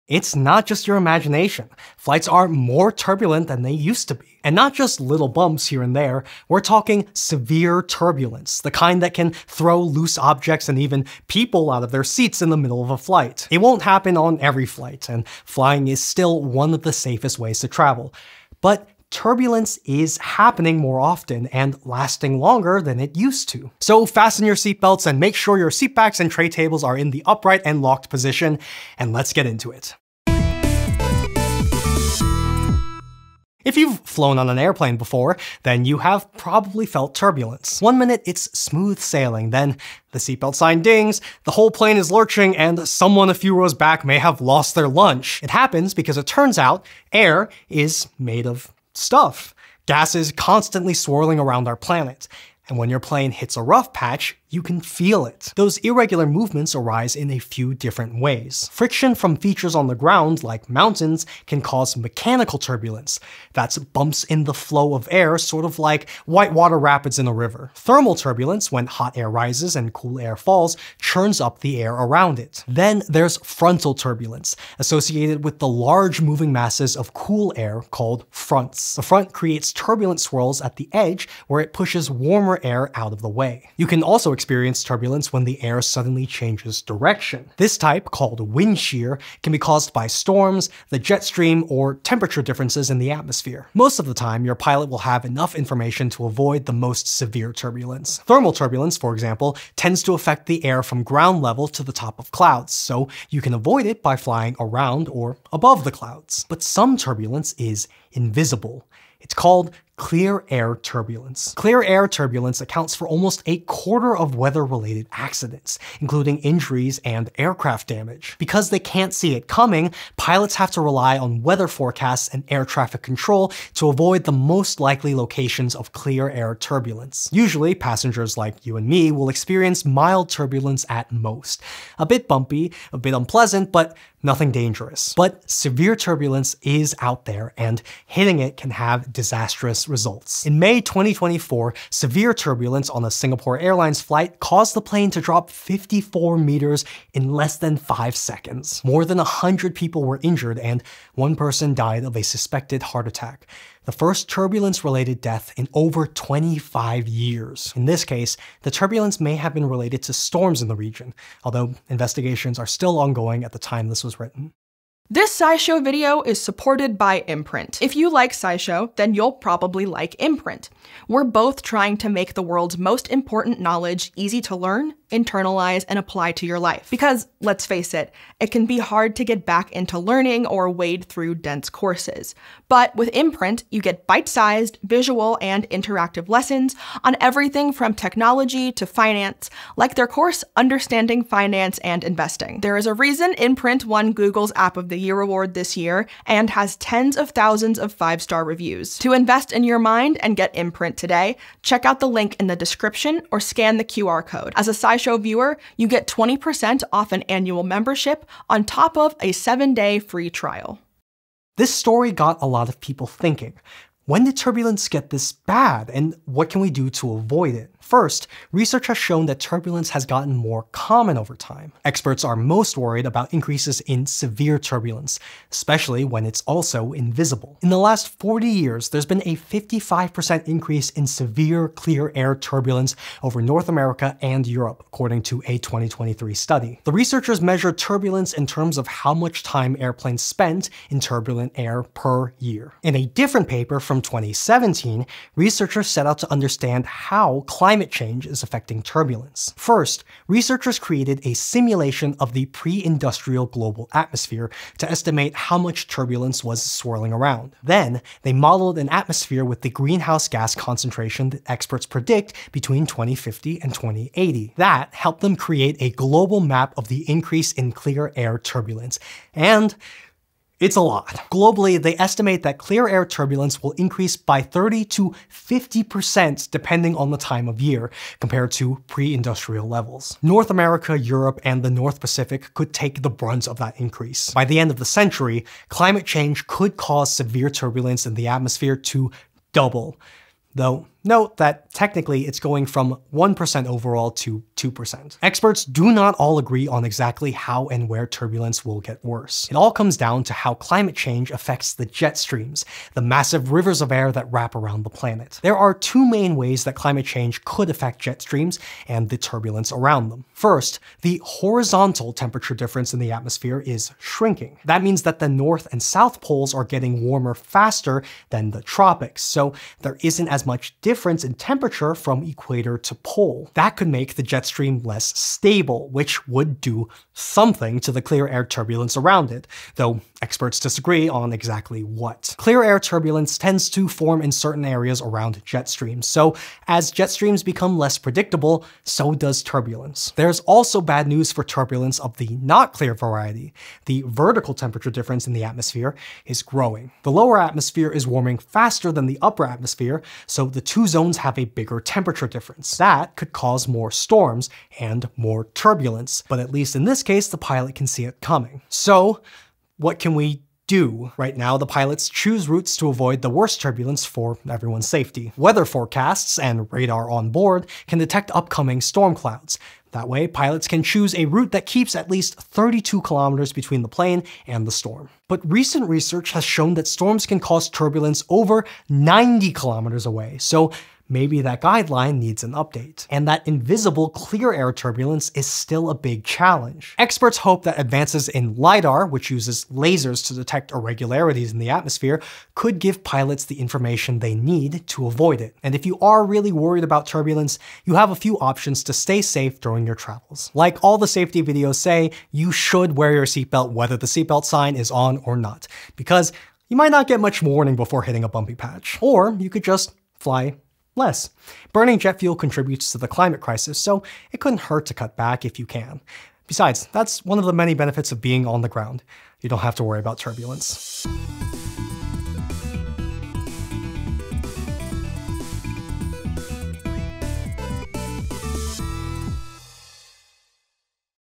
Yes, Flights ARE Bumpier Now (And They're Getting Worse)
It's not just your imagination. Turbulence on airplane flights is getting worse, including severe turbulence that could cause injuries. The reason is the climate crisis -- here's how.